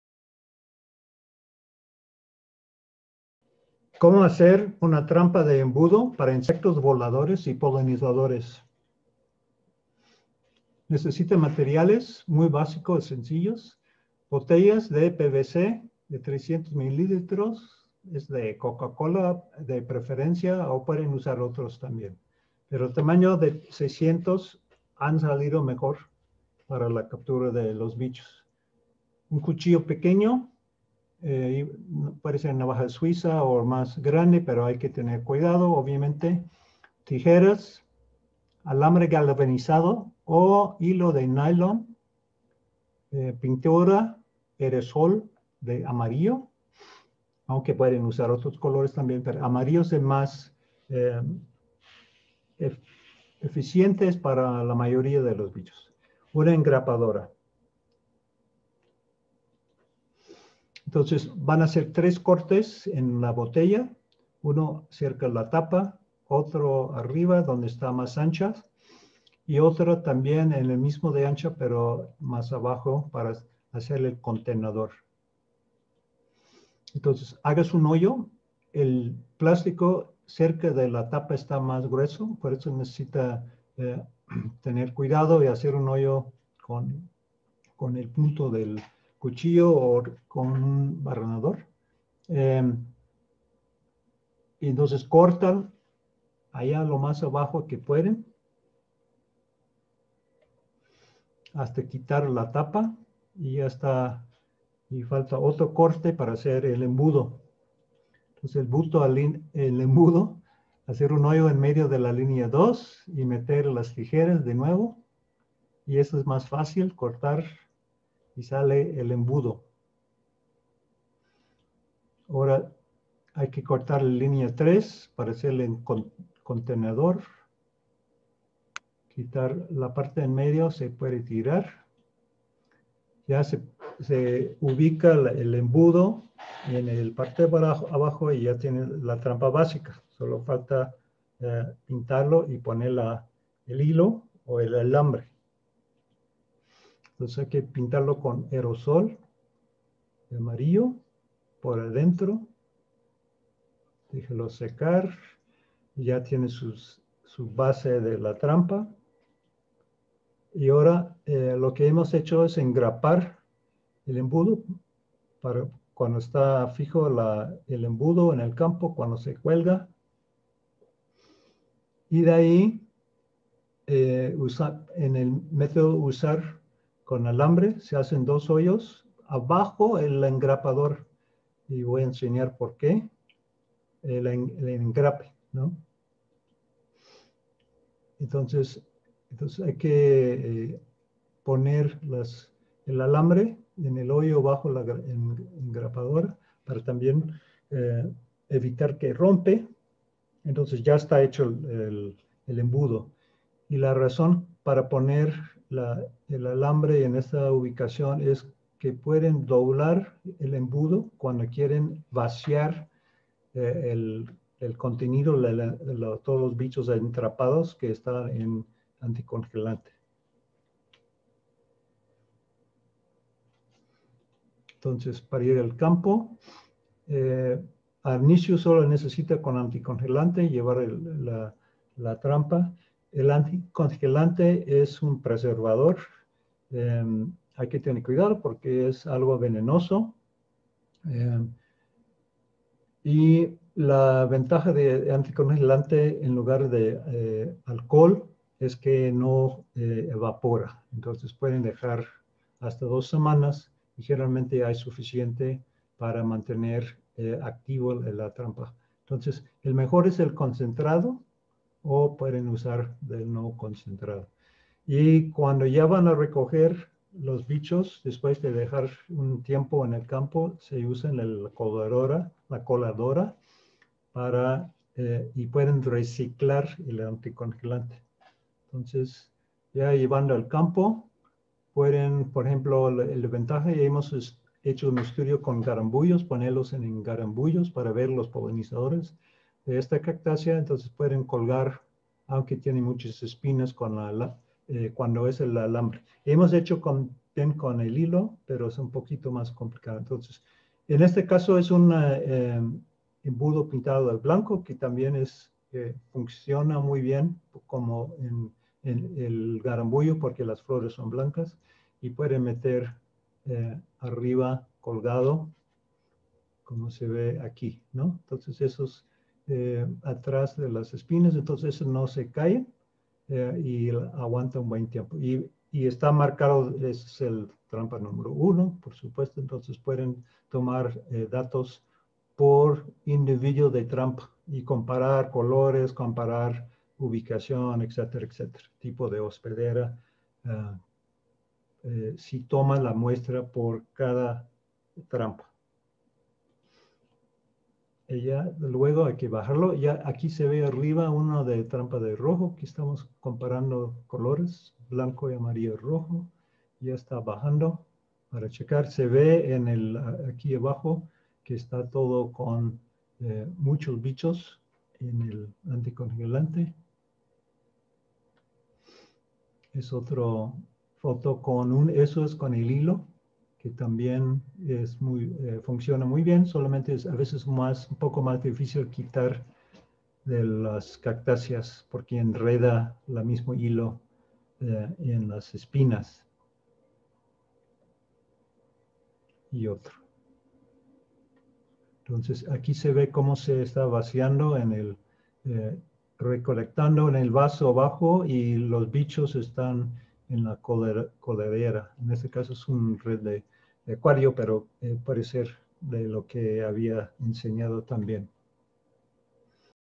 TRAMPA PARA EL MONITOREO DE POLINIZADORES | TUTORIAL
En este vídeo el Dr. Robert W. Jones (Prof. investigador de la Facultad de Ciencias Naturales de la Universidad Autónoma de Querétaro, México), nos comparte la elaboración práctica y sencilla para la elaboración de trampas que nos servirán en el estudio y monitoreo de insectos polinizadores, utilizando materiales reciclados y de fácil acceso.

Si te gusto el vídeo, puedes compartirlo con tus amigos o compañeros.

Recuerden, ¡Dejemos que el conocimiento científico fluya!